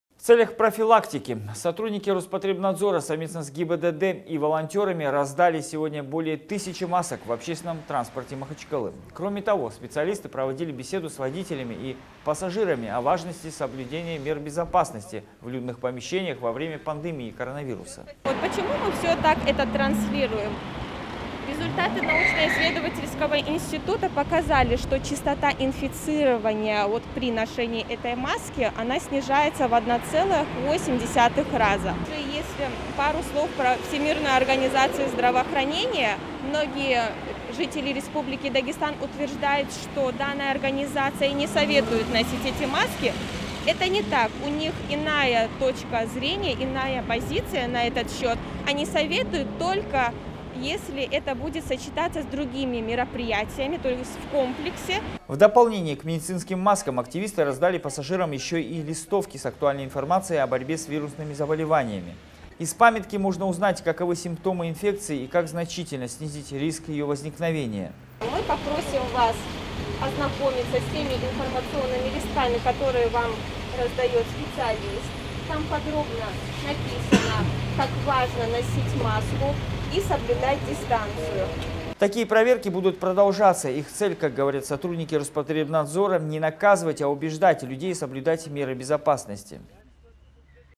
«Защити себя». Более тысячи масок раздали в общественном транспорте Махачкалы
В целях профилактики. Сотрудники Роспотребнадзора совместно с ГБДД и волонтерами раздали сегодня более тысячи масок в общественном транспорте Махачкалы.

Кроме того, специалисты проводили беседу с водителями и пассажирами о важности соблюдения мер безопасности в людных помещениях во время пандемии коронавируса.

Екатерина Комаревцева – старший специалист Роспотребнадзора РД
В дополнение к медицинским маскам активисты раздали пассажирам еще и листовки с актуальной информацией о борьбе с вирусными заболеваниями. Из памятки можно узнать, каковы симптомы инфекции и как значительно снизить риск ее возникновения.

Такие проверки будут продолжаться. Их цель, как говорят сотрудники Роспотребнадзора, не наказывать, а убеждать людей соблюдать меры безопасности.